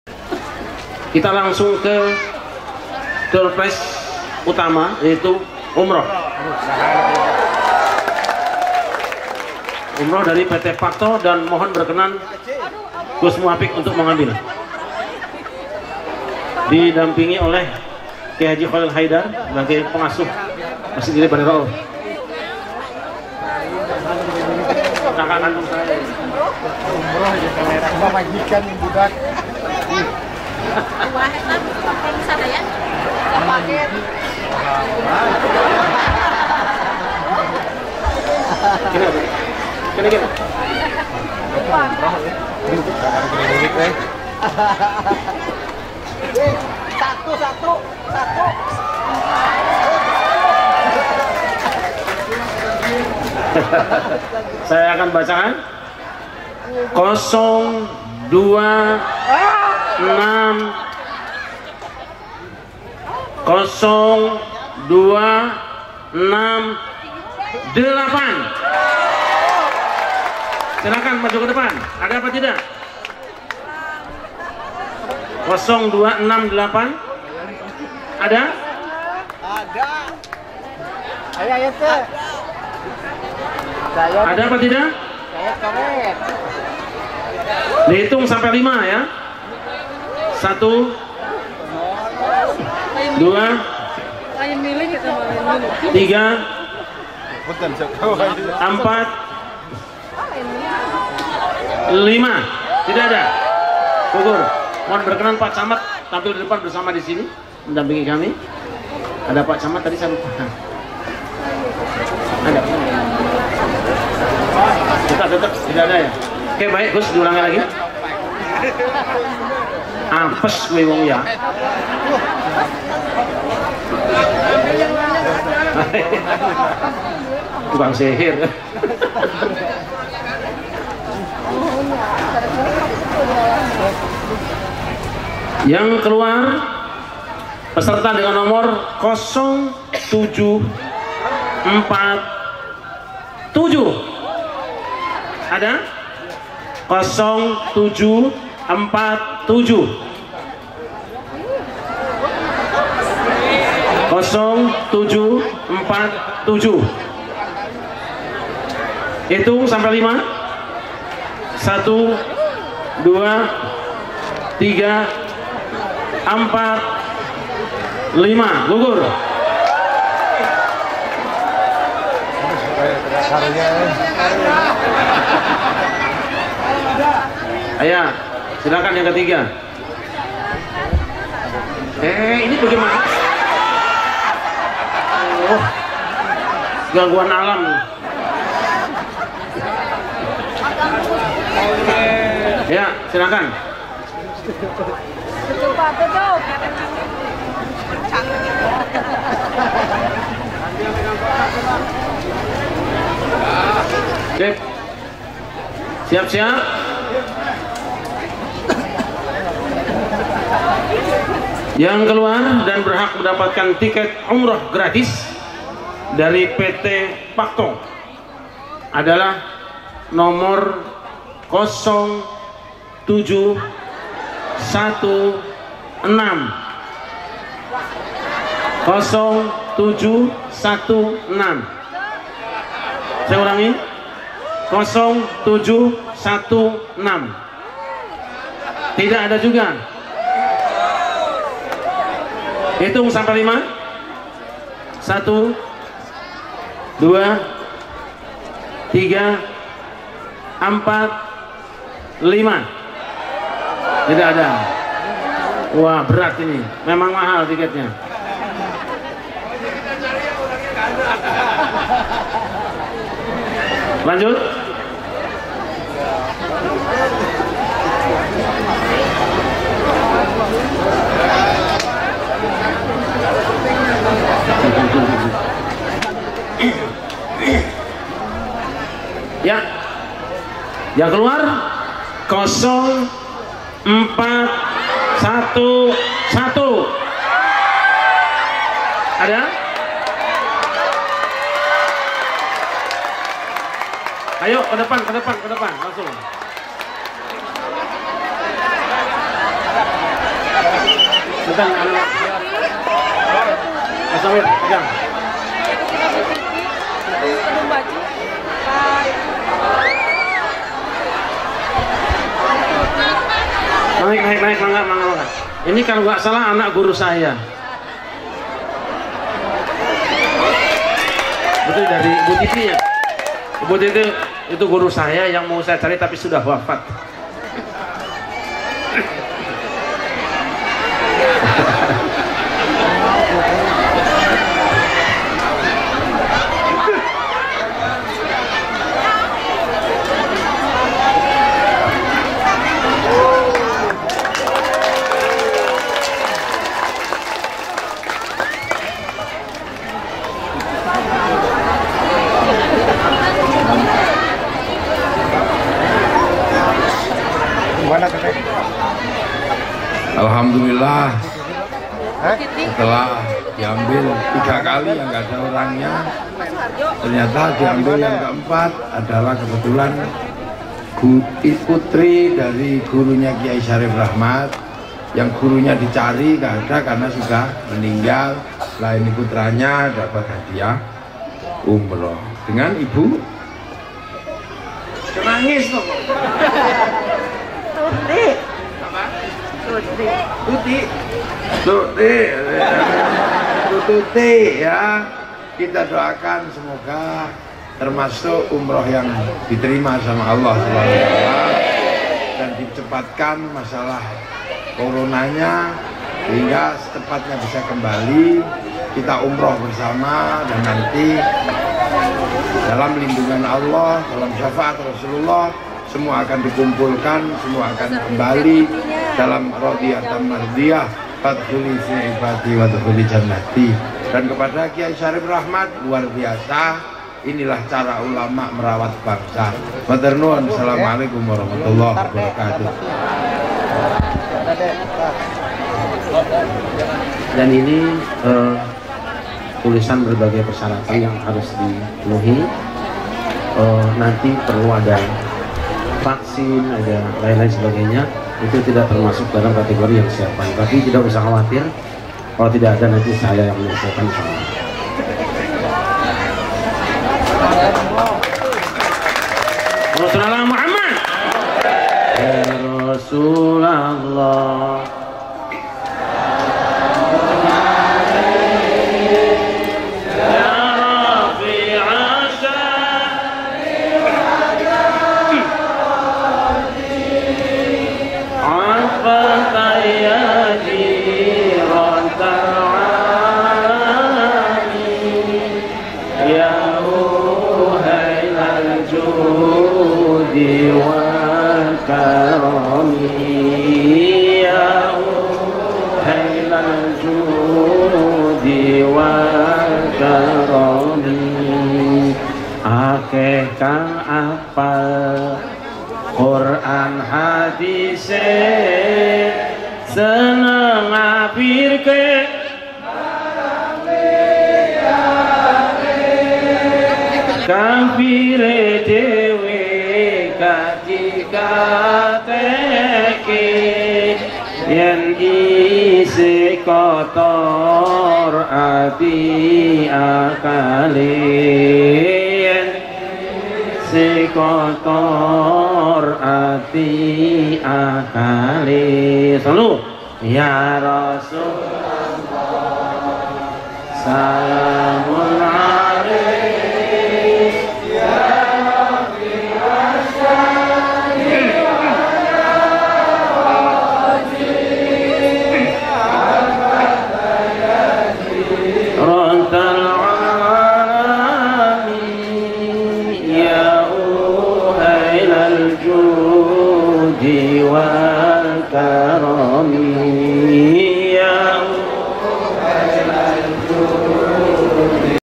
Gus Muwafiq Ambil Hadiah Umroh dari PT. Pacto Untuk Warga Ciamis | Haul Raden Romo Ke-35
Meneguhkan Keislaman Dalam Bingkai Kebangsaan